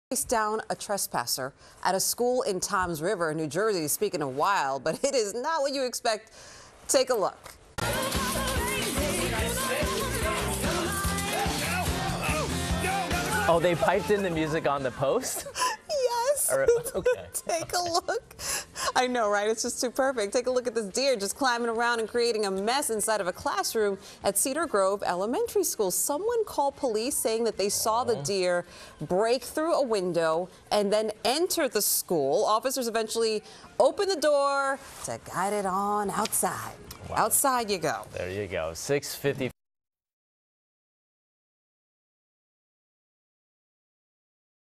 Police chase deer through elementary school in NJ
Police chased a deer through an elementary school in Toms River, New Jersey.